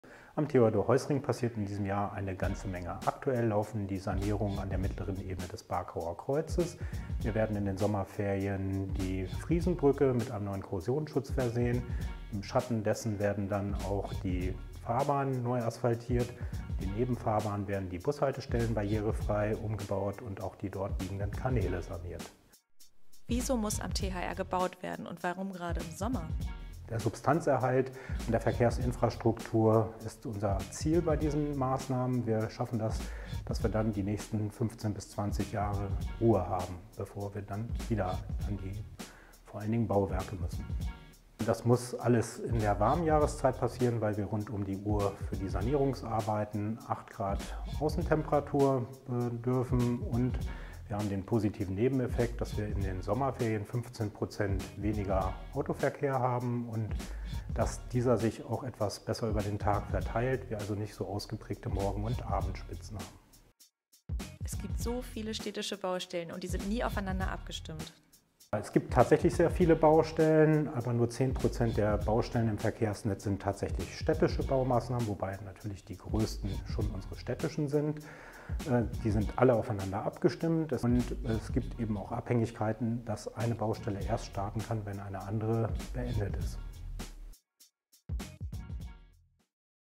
Was ist los am Theodor-Heuss-Ring (THR)?
Damit wir alle den THR mit dem Auto weiterhin sicher nutzen können, muss in einigen Bauabschnitten in diesem Jahr vieles neu gemacht werden: am Barkauer Kreuz, an der Friesenbrücke und in den Nebenstraßen. Wenn irgendwo Straßen saniert werden, schaut das Tiefbauamt auch direkt, was im Zuge dieser Arbeiten noch erneuert werden kann. Am THR sind es die Asphaltdecken, Kanäle und auch Bushaltestellen, die barrierefrei gemacht werden.

Wir wissen: Baustellen stören und sind ein Zeitfresser. Aber nach einigen Jahren müssen gerade viel befahrene und belastete Straßen neu gemacht werden.

Übrigens gibt es jedes Jahr eine Baustellenplanung - einige Baustellen können erst beginnen, wenn andere aufhören und wieder andere können nicht zeitgleich stattfinden. Es steckt also System hinter den Abläufen der städtischen Baustellen. Nur 10 Prozent aller Baustellen in Kiel sind städtisch, der Rest sind private Baustellen, von den Stadtwerken oder für den Glasfaserausbau.

und alles zu den anderen Baustellen in Kiel findet Ihr in unserer Baustellen-Karte.